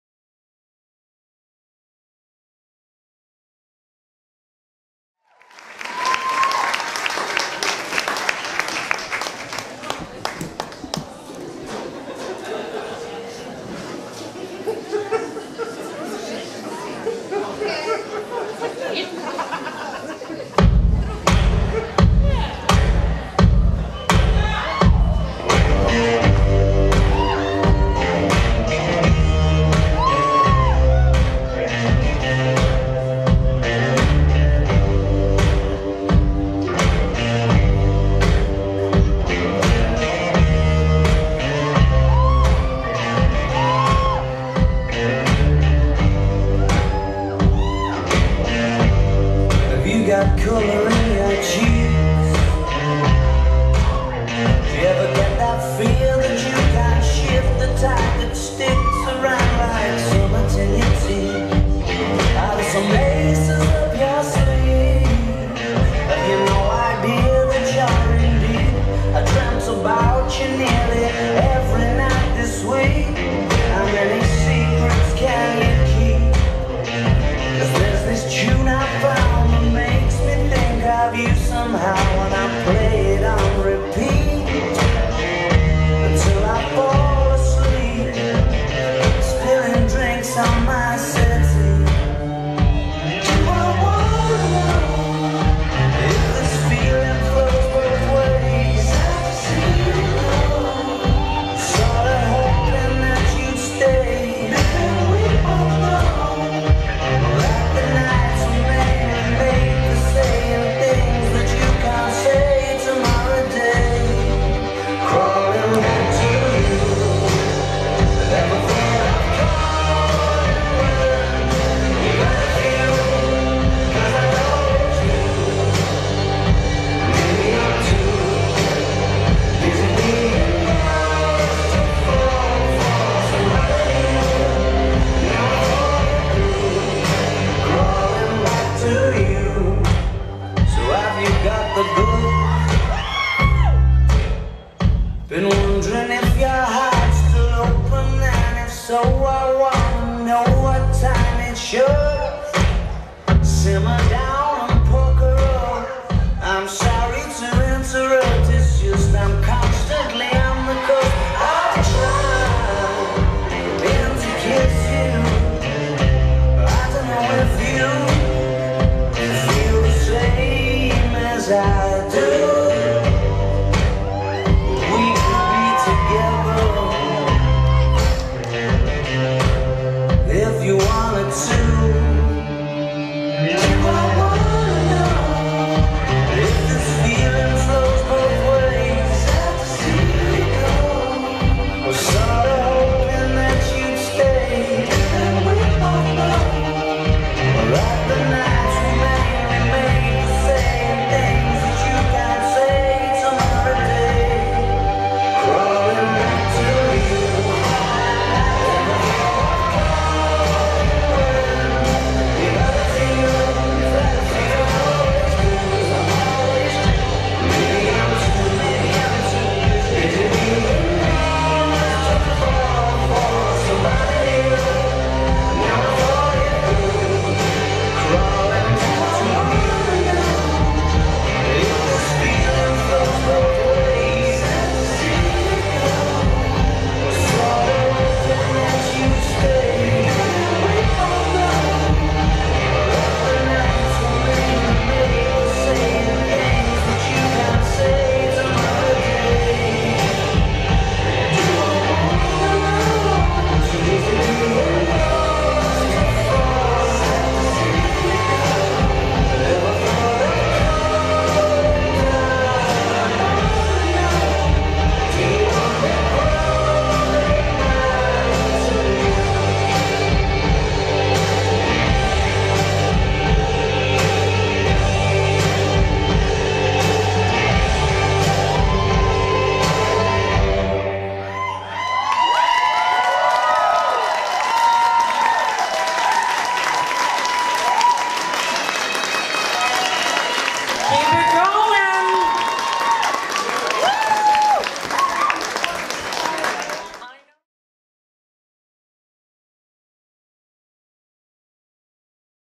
Toronto Burlesque Festival 2015 - Champagne Smash - Tanya Cheex
This rock and roll shakedown is the last hurrah of the festival. This full tilt, farewell evening will have you on your feet, loudly cheering on the leading edge of neo-burlesque.